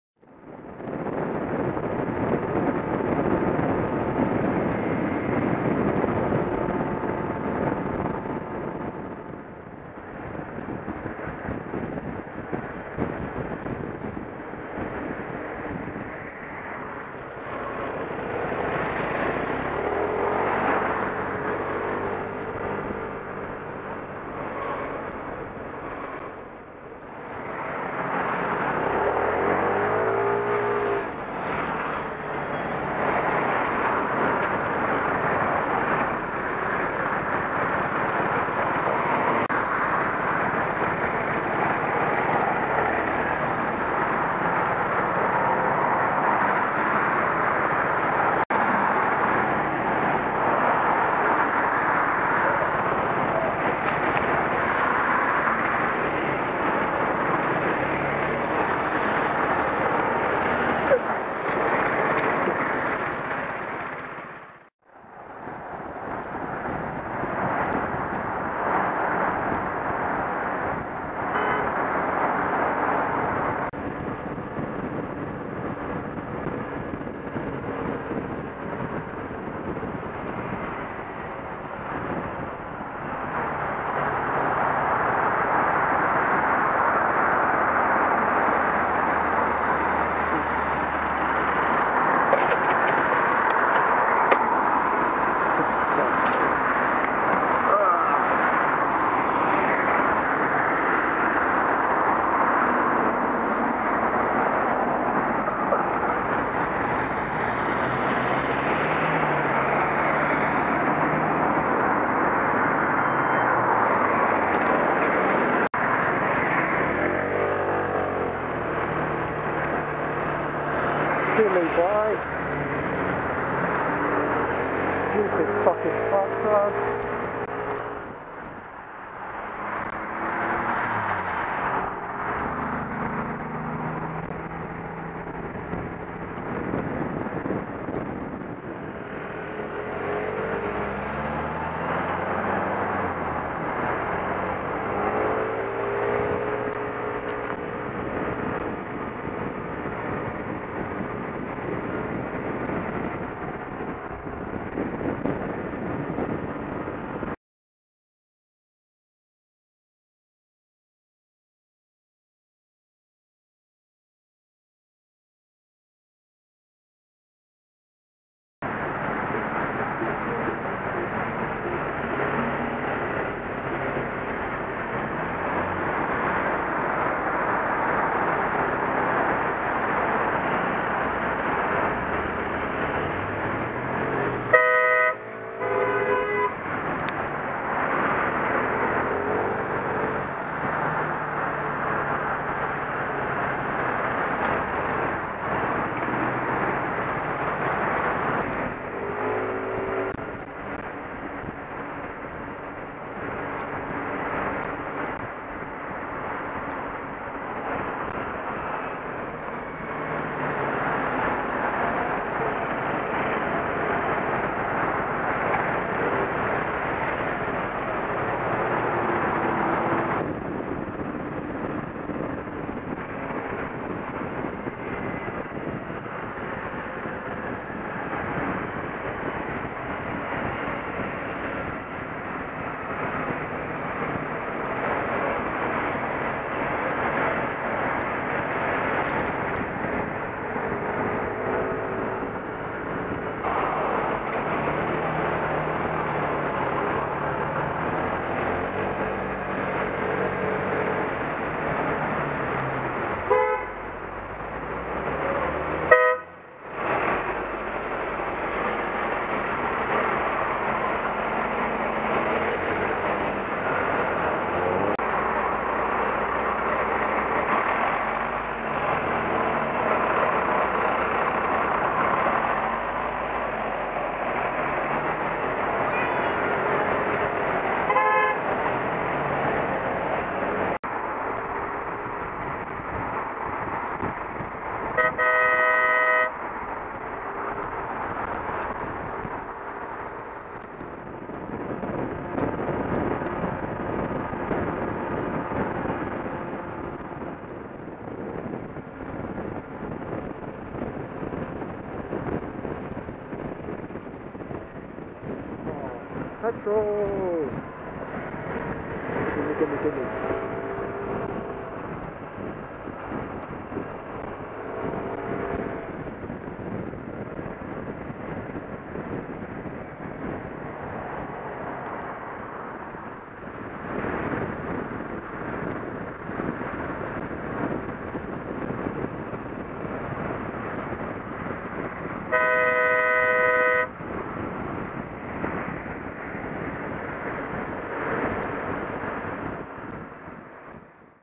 #74 - 'Tis The Season
Pre-Christmas road trip from Brum to London featuring close calls, road rage and random encounters... all in sub-zero temperatures. Dontcha just love winter? :|

This video is for educational and entertainment purposes because learning is fun.

Filmed on a ContourROAM 2.